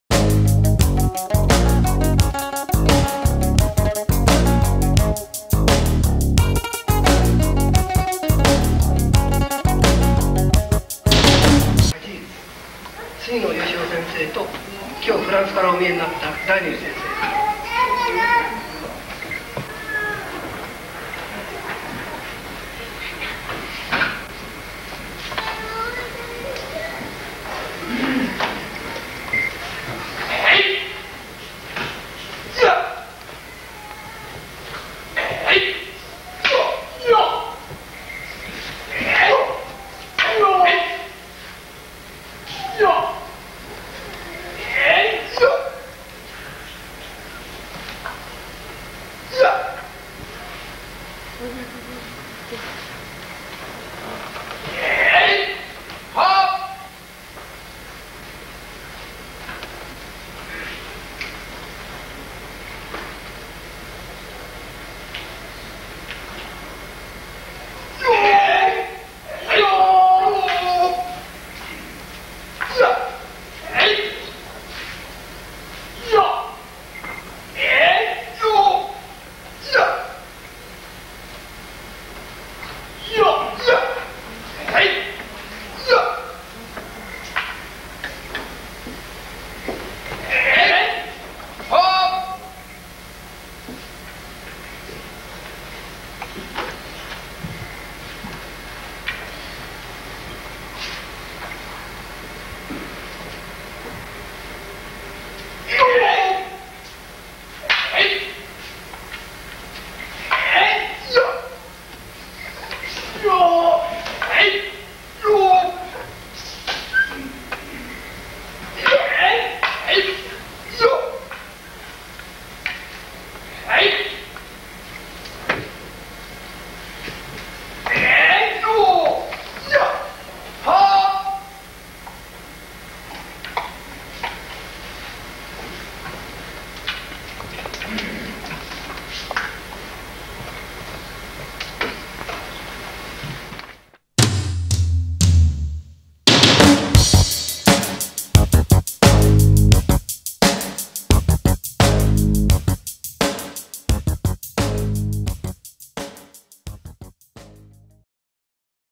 Katori Shinto Ryu - Kenjutsu kata Demonstration - Yoshio Sugino sensei
Yoshio Sugino sensei demonstration of katori Shinto Ryu kenjutsu kata at the 9th anniversary celebration of the International Budo Federation in Japan.

Démonstration de kata de kenjutsu de l'école Katori Shinto Ryu par sensei Yoshio Sugino lors de la cérémonie du 9ème anniversaire de la Fédération Internationale de Budo.

Retrouvez tout le programme technique de notre école sur les DVD officiels qui font référence pour les examens de grade

Note: la qualité vidéo reste moyenne car le film original a été tourné en VHS. Thank you to Sato Tsugiyoshi san for the video.